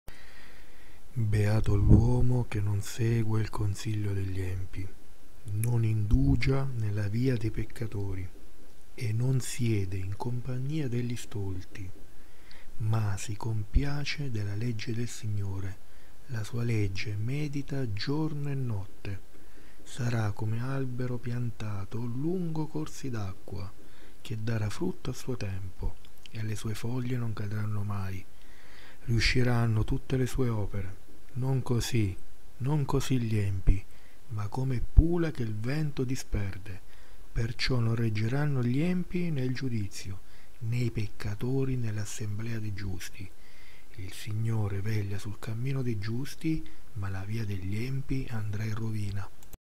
Salmo 1 Le due vie.
[1] Beato l'uomo che non segue il consiglio degli empi,
non indugia nella via dei peccatori
e non siede in compagnia degli stolti;

[2] ma si compiace della legge del Signore,
la sua legge medita giorno e notte.

[3] Sarà come albero piantato lungo corsi d'acqua,
che darà frutto a suo tempo
e le sue foglie non cadranno mai;
riusciranno tutte le sue opere.

[4] Non così, non così gli empi:
ma come pula che il vento disperde;

[5] perciò non reggeranno gli empi nel giudizio,
né i peccatori nell'assemblea dei giusti.

[6] Il Signore veglia sul cammino dei giusti,
ma la via degli empi andrà in rovina.